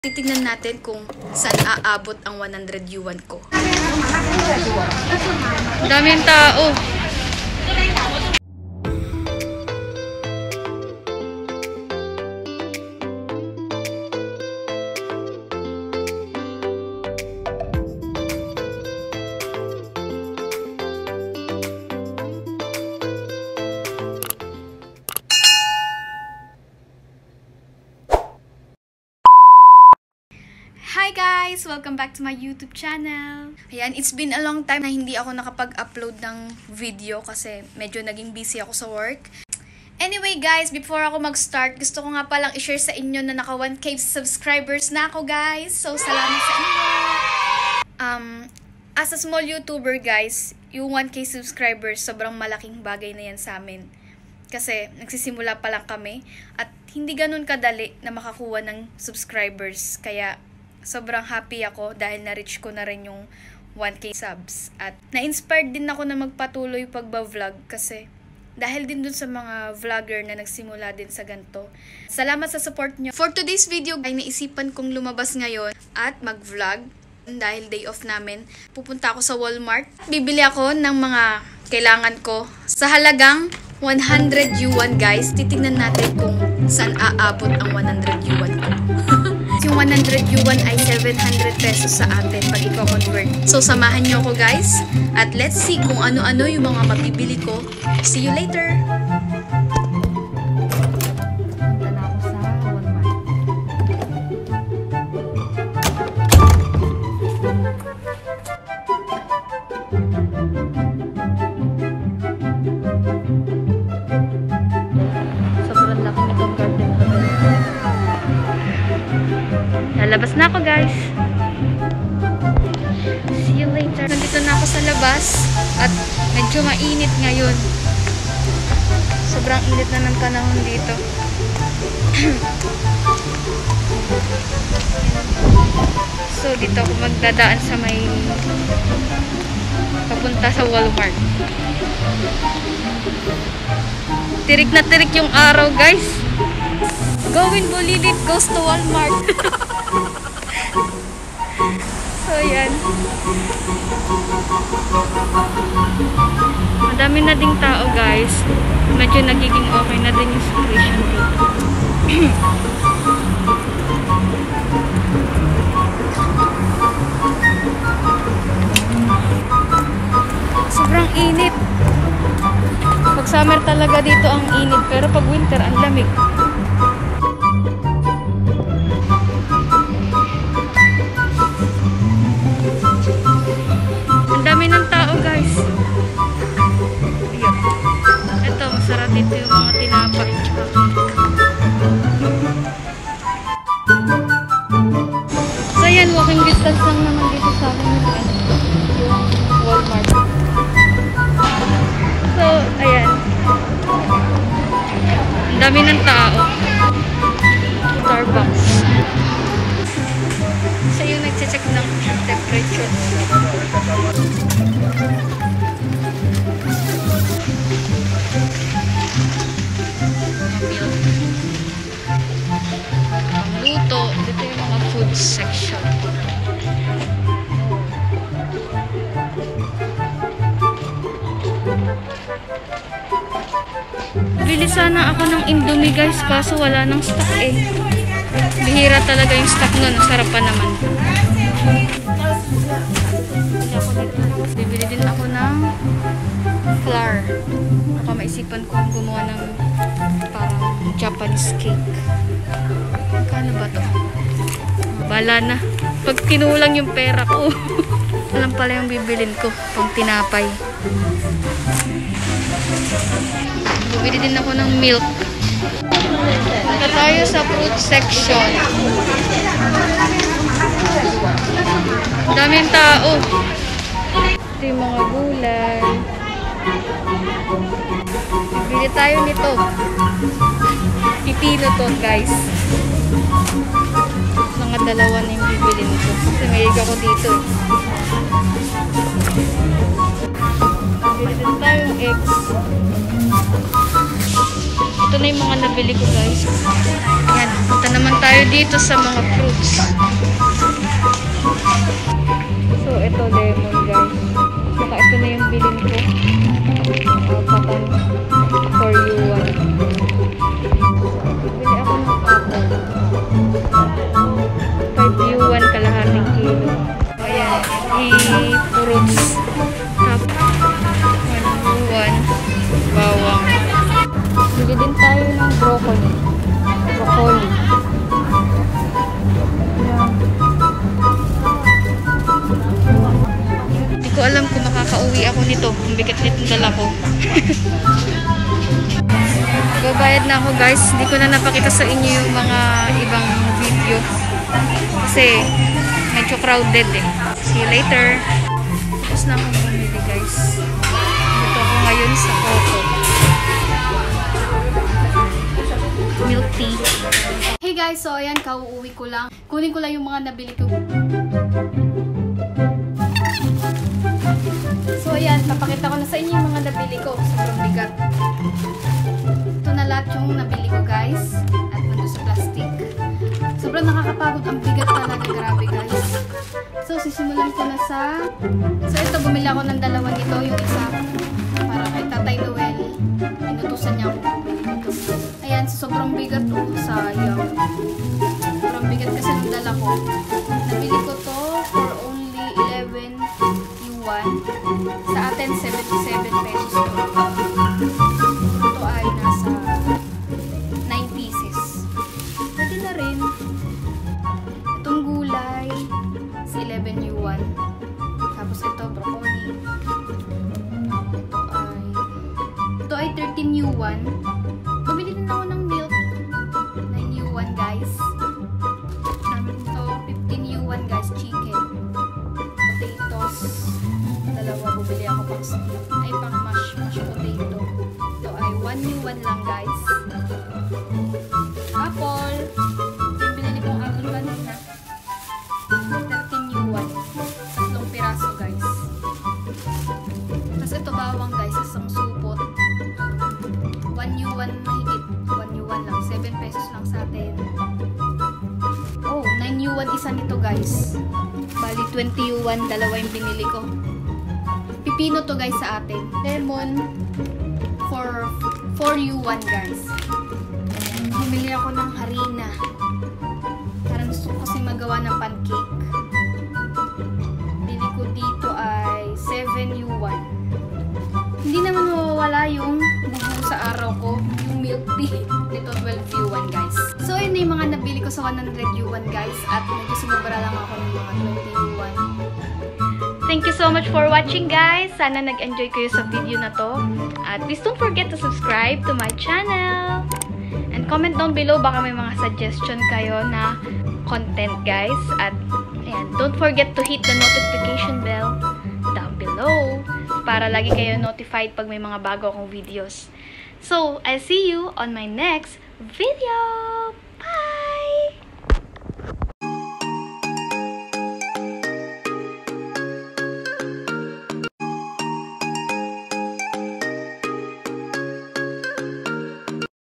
100RMB|700 pesos WORTH OF FOOD GROCERY in CHINA
In this video, I went to Walmart  and
i'll show you the current situation here in China
Afterlockdown

Mas mura ang bilihin sa palengke nila guys. Walmart edition to
Next time sa wet market ng china tayo pupunta ! 🇨🇳